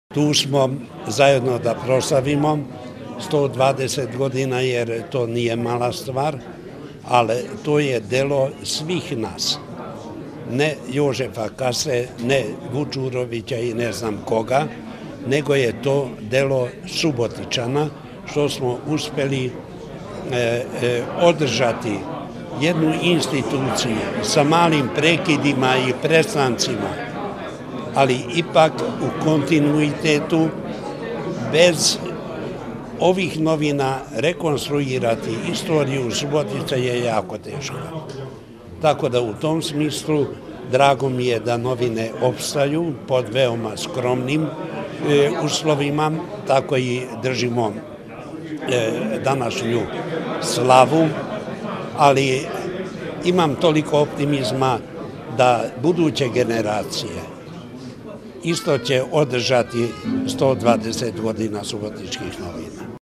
120 godina "Subotičkih novina"
„Subotičke novine" juče su obeležile 120 godina postojanja. Prvi broj „Subotičkih novina", tada nedeljnika za misne, obće stvari, prosvitu, zabavu i gazdinstvo,  izašao je 1.januara 1893. godine.

U početku, list je izlazio nedeljom a od aprila 1896. godine svake subote, od kada postaje glasilo za politiku, prosvitu i privredu. Pisan je novoštokavskim govorom Bunjevaca u Bačkoj. Zbog oštrih kritika vlasti od strane vlasnika, odgovornog urednika i izdavača Mladena Đorđa Karanovića, „Subotičke novine" su ugašene 1898.godine, ali su se posle Prvog svetskog rata ponovo pojavile. Novine su tokom dvanaest decenija menjale vlasnike, imena, uređivačku politiku, prolazile su kroz uspone i padove, lepe ali i teške periode. Obeležavanju jubileja „Subotičkih novina" prisustvovao je i zamenik gradonačelnika Subotice, Tomilsav Veljković. On je naglasio da su "Subotičke novine" list svih Subotičana i da tako treba da bude i u budućnosti.Na svečanosti su predstavljeni i likovni i literalni radovi učenika koji su učestvovali na konkursu "Subotičkih novina" i pisali ili slikali i crtali na temu novinarstva, a zahvalujući Gradskoj biblioteci izloženi su i  faskimili stranica "Subotičkih novina" kroz istoriju dugu 120 godina.